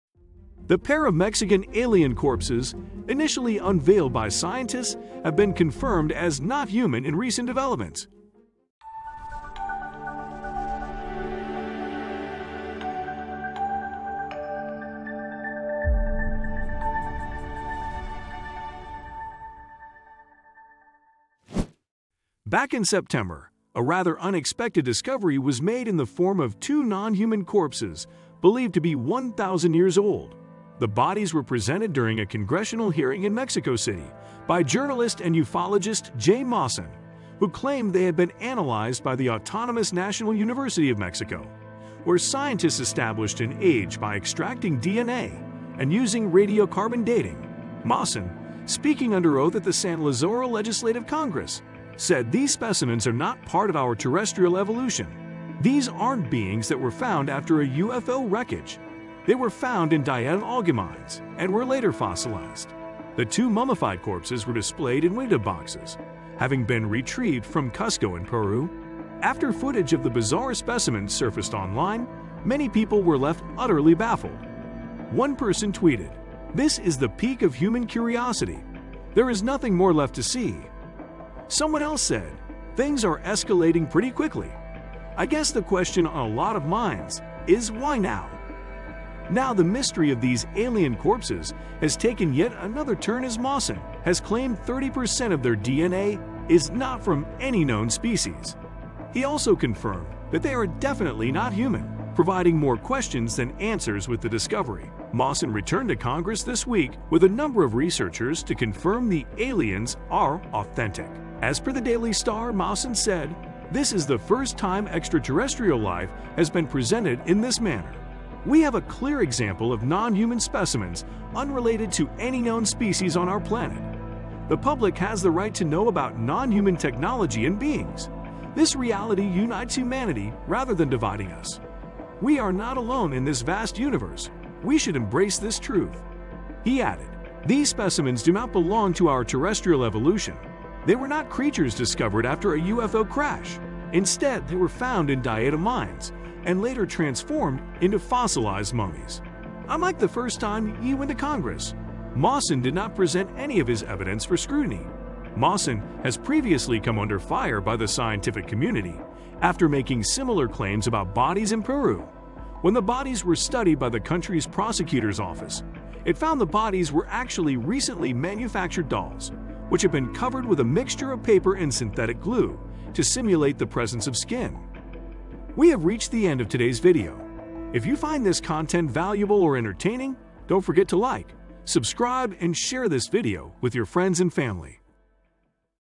Shocking Discovery: Scientists Find Non-Human DNA in Alien Remains
In a shocking discovery, scientists have uncovered non human DNA in alien remains!

This discovery has raised many questions about the origin of these aliens, and whether or not they are of human origin.